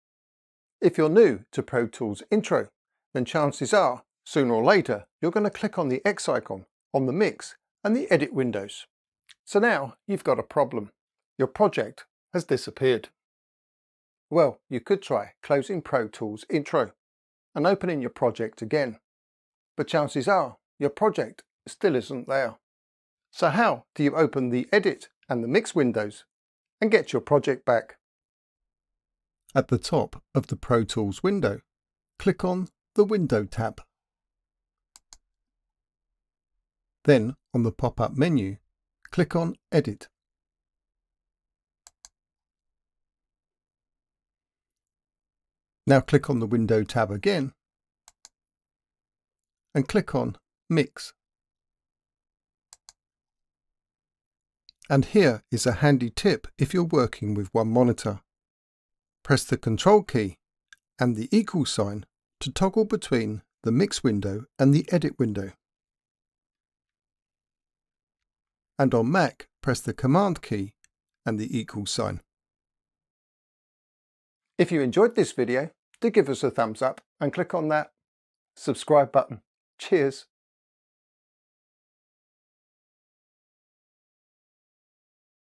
How to open the Edit Window in Pro Tools
Opened your project but nothing is there? Here is how to open the Edit Window and the Mix Window and get your project back. Plus, how to toggle between the Edit and Mix Windows.
It's a common problem for new Pro Tools users, you closed the Edit Window and now you have no idea how to open it again. Or you open a project you saved earlier only to find the screen is blank... it looks like your project has gone... but usually you just need to open the Edit Window.